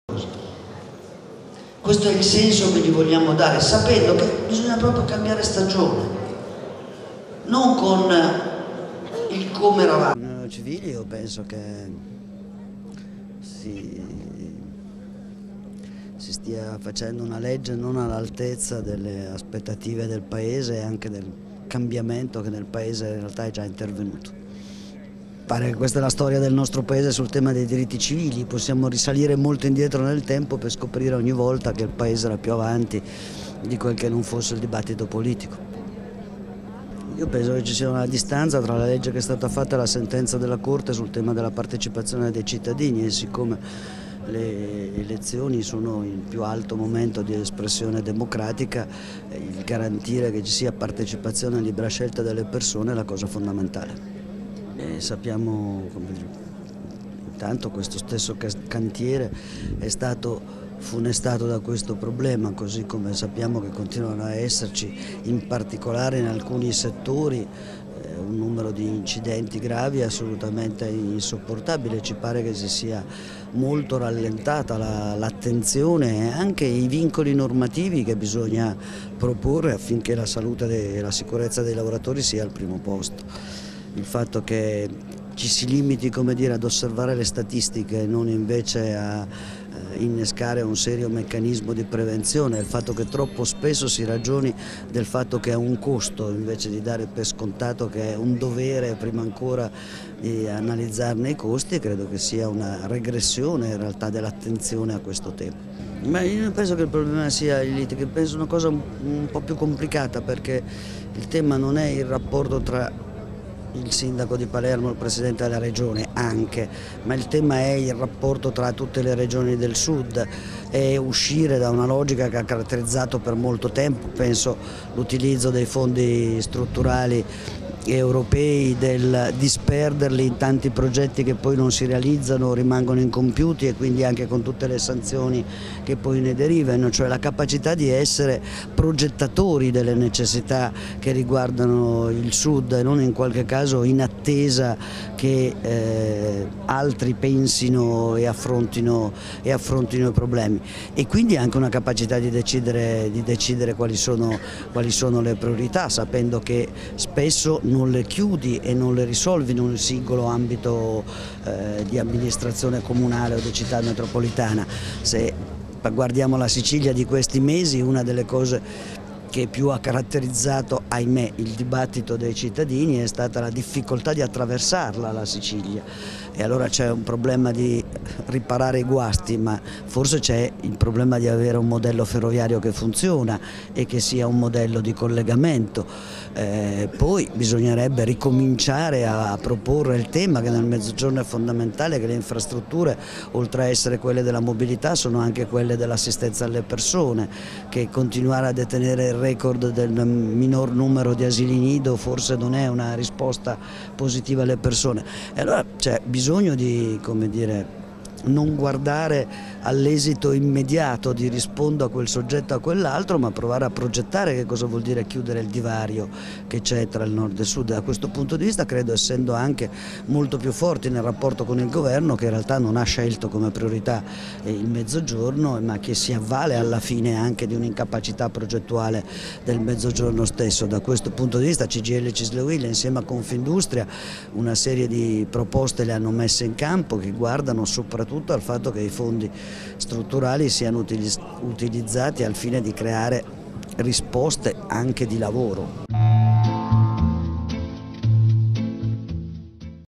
Susanna Camusso a Palermo, basta precari e morti sul lavoro
E’ giunta a Palermo di buonora Susanna camusso il segretario nazionale della Cgil per partecipare a una assemblea sulla Carta dei Diritti Universali dei lavoratori all’interno del deposito del tram di via Castellana, nell’officina dell’Amat intitolata a Giovanni Mannino, l’operaio di 41 anni che perse la vita nell’aprile del 2013 mentre lavorava al cantiere del tram.